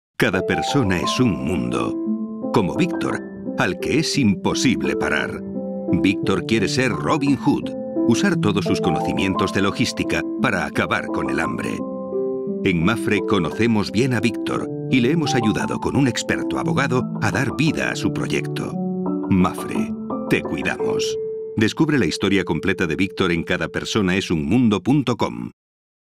Victor vive en la eterna juventud. MAPFRE teCuidamos - MAPFRE España
Conoce la historia de Victor, un hombre que quiere poner, a disposición de los demás, toda su experiencia laboral, para él nunca es tarde para ser quién quieres y no hay límites de edad para hacer lo que te apasiona y para ello cuenta con la ayuda de MAPFRE teCuidamos.